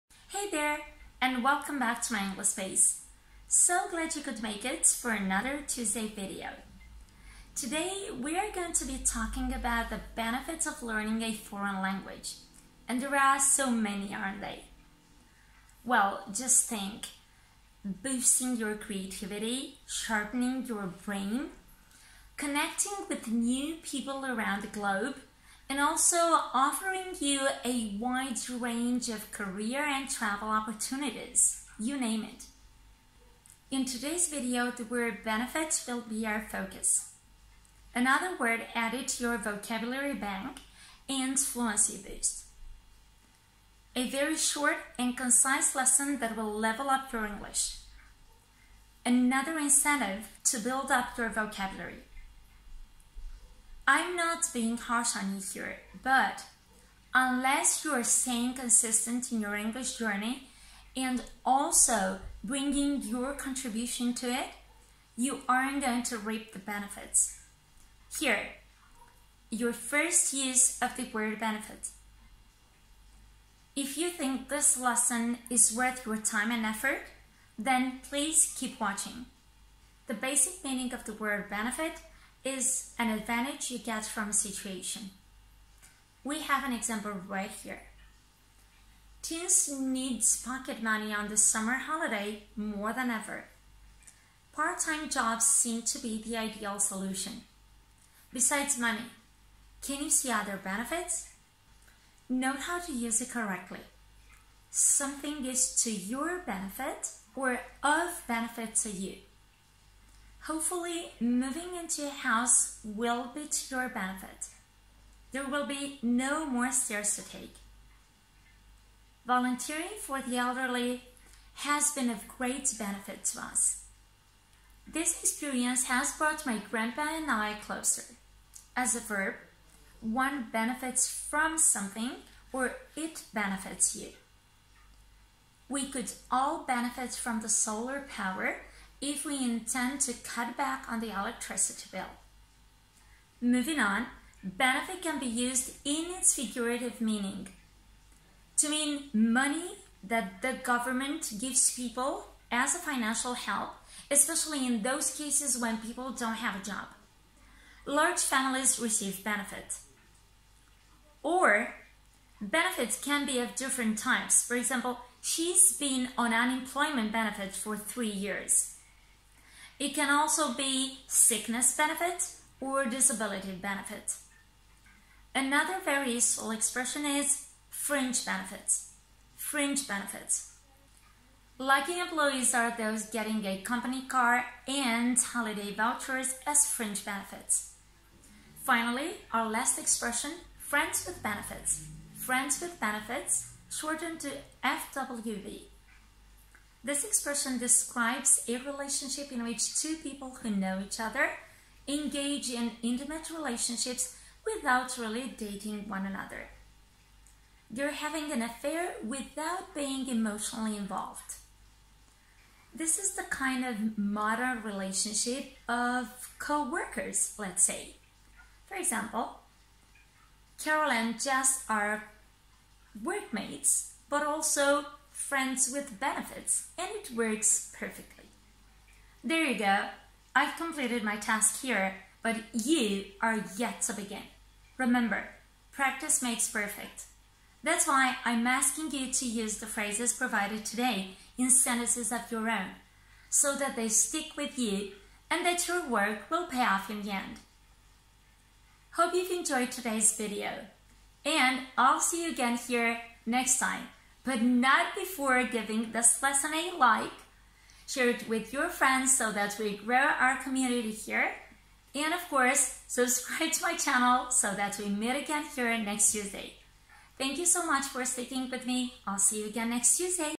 Expand your vocabulary| Use BENEFIT correctly
Another chance to benefit from my explanations and practical examples, now right with the word "benefit"!
      Be sure to be all ears and not only follow through the grammar tips, but also the example sentences so that you can start making your own afterwards.
Thank you so much - I'll talk to you in the comments!

FANCY SOME MORE?